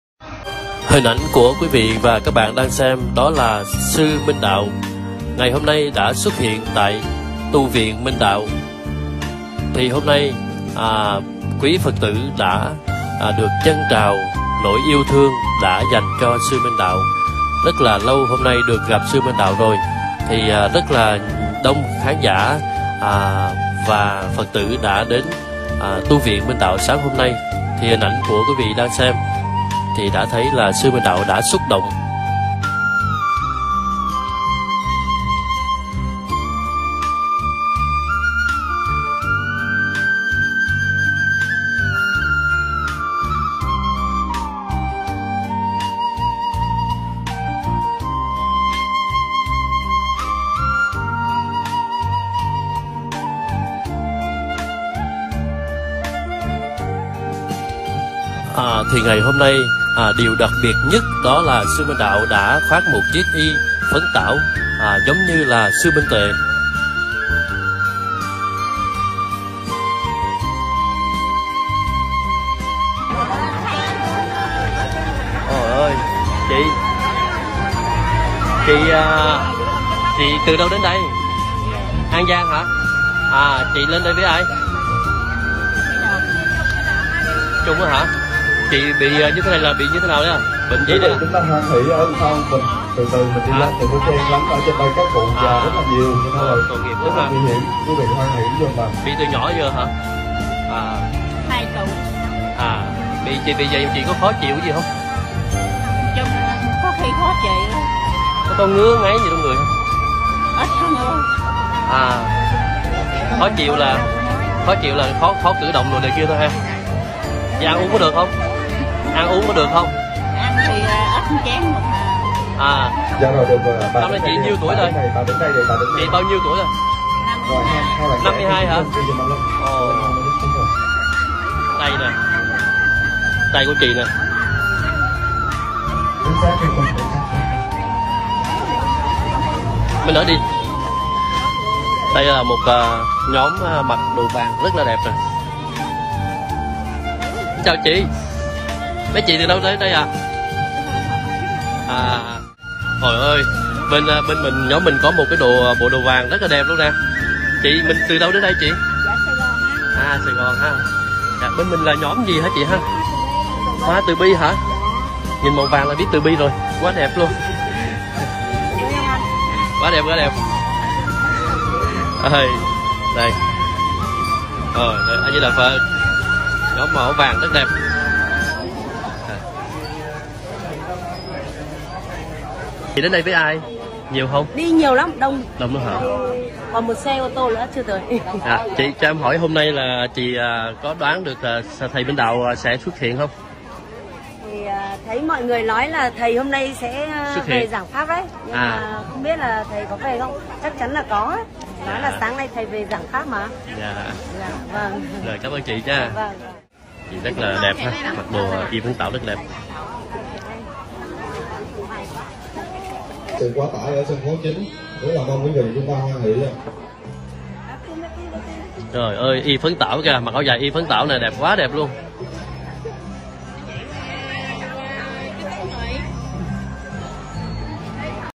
phật tử đến tu viện thầy Minh đạo quá đông
11/8 thầy Minh đạo mới nhất thichminhtue #minhdao